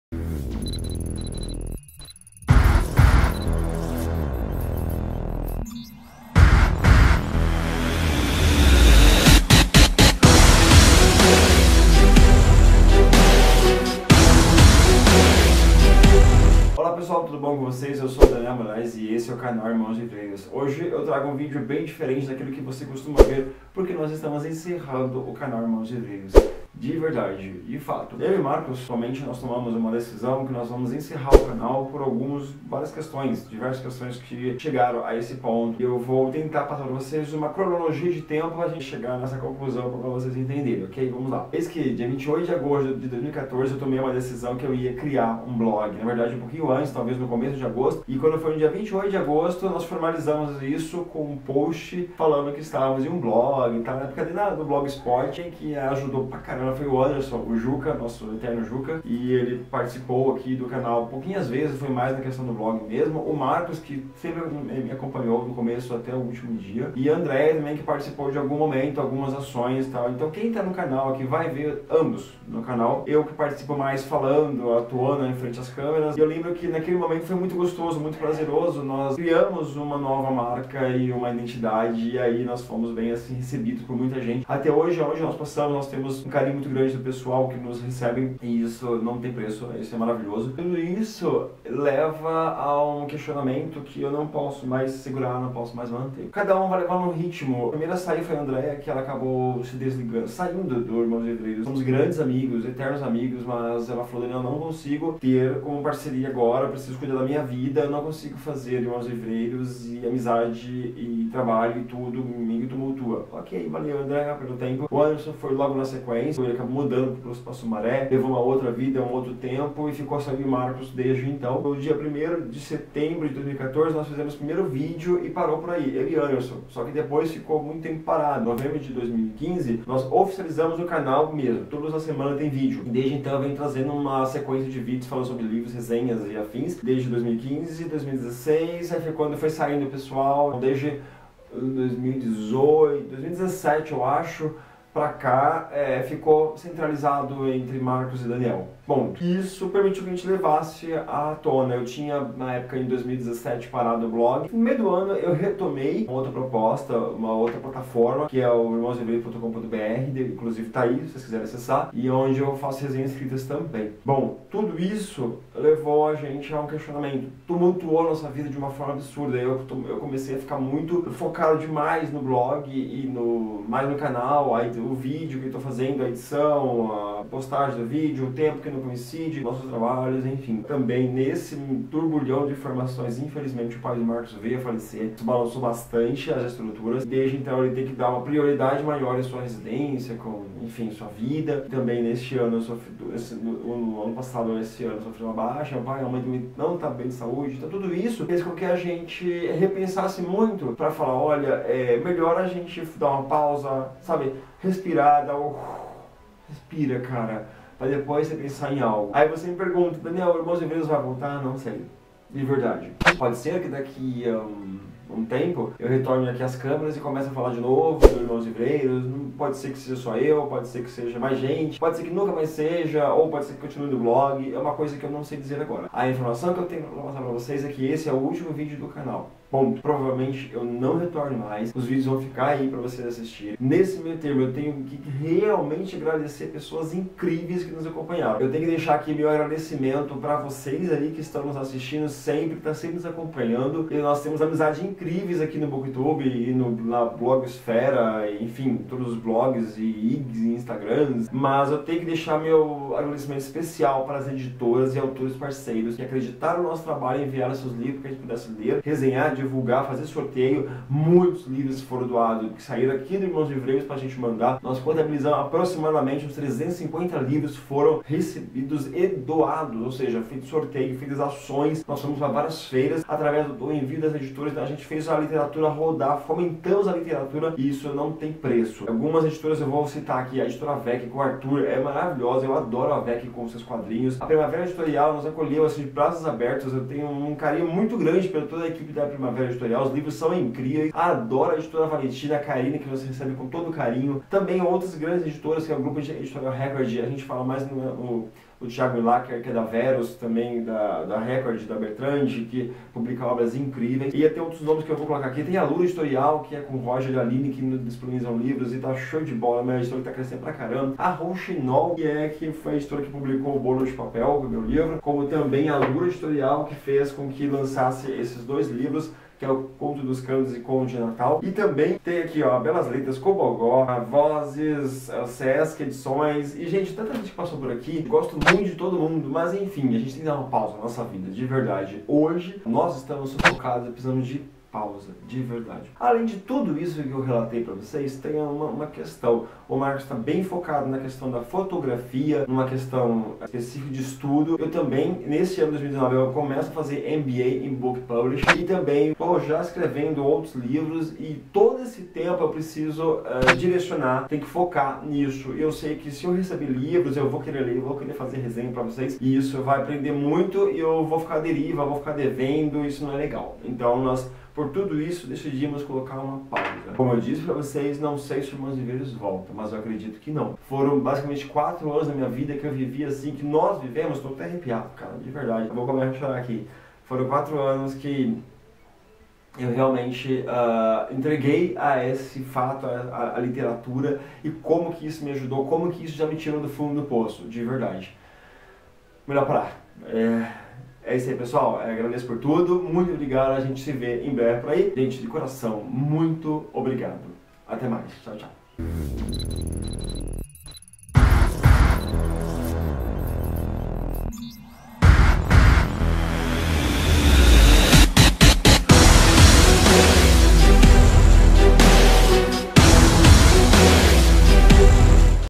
FIM DO CANAL ★ IRMÃOS LIVREIROS
O canal Irmãos Livreiros, após 4 anos, chega ao fim.
Por isso, neste vídeo, me despeço de todos que nos acompanharam desde o início, e levo comigo cada um de vocês para onde eu for, afinal, vocês são incríveis!
Sentiremos saudades dos momentos que aqui passamos juntos, mas esse é o momento de seguir outros rumos.
Nos veremos por aí! =)

PS:  ONDE ENCONTRAR OS MEUS LIVROS:

► BODAS DE PAPEL
► O CANTO DOS CONTOS
► CONTOS DE NATAL

MUITO OBRIGADO À TODOS VOCÊS!
VOCÊS SÃO INCRÍVEIS! =)